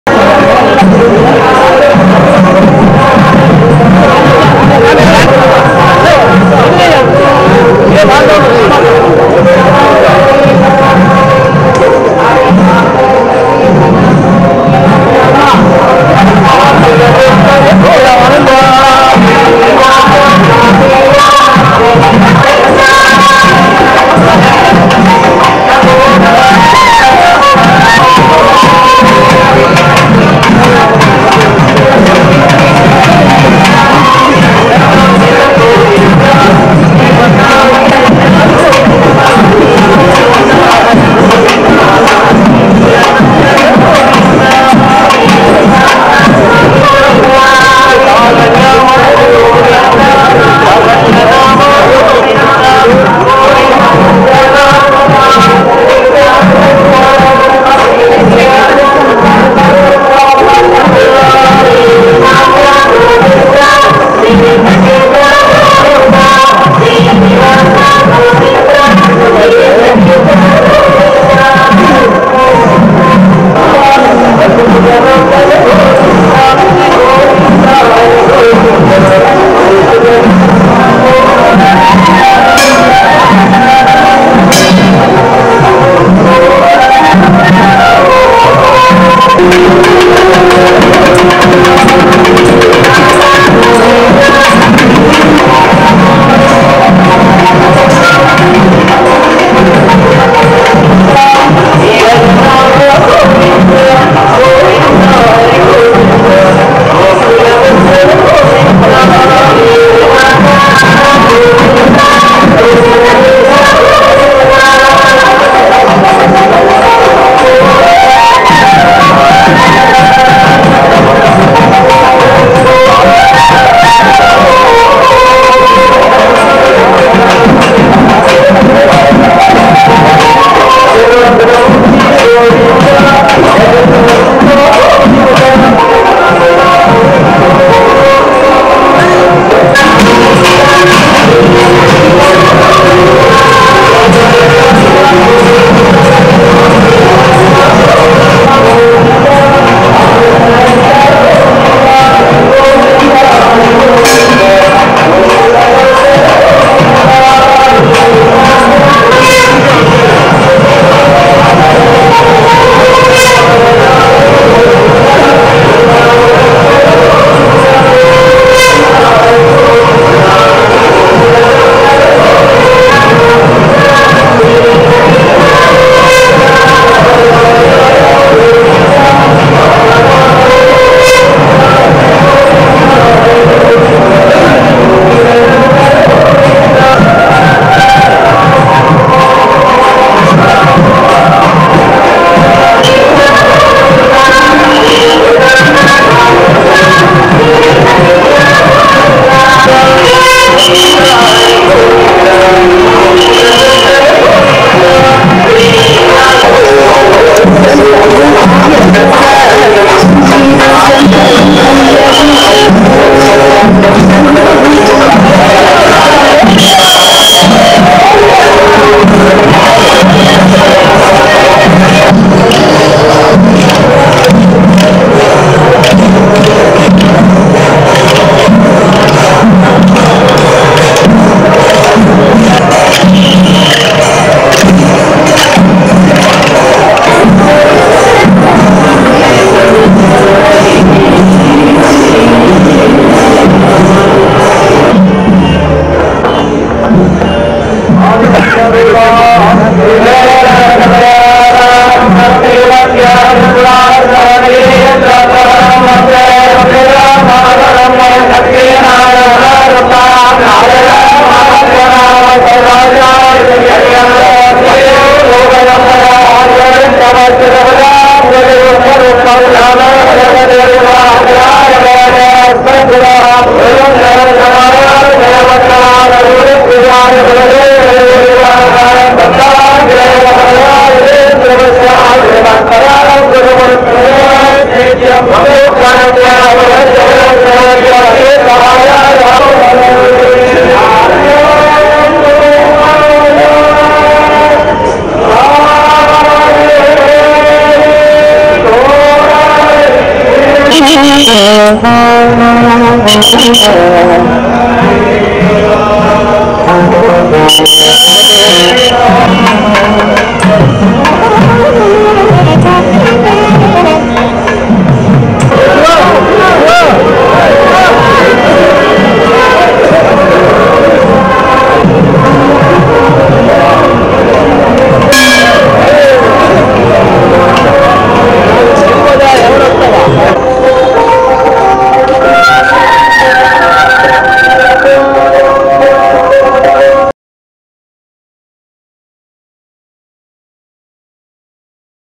గోవిందరాజస్వామి బ్రహ్మోత్సవాలలో హనుమంత వాహనంపై స్వామివారు
Swami varu on Hanumantha Vahanam in Govindaraja Swamy Brahmotsavam, Tirupathi.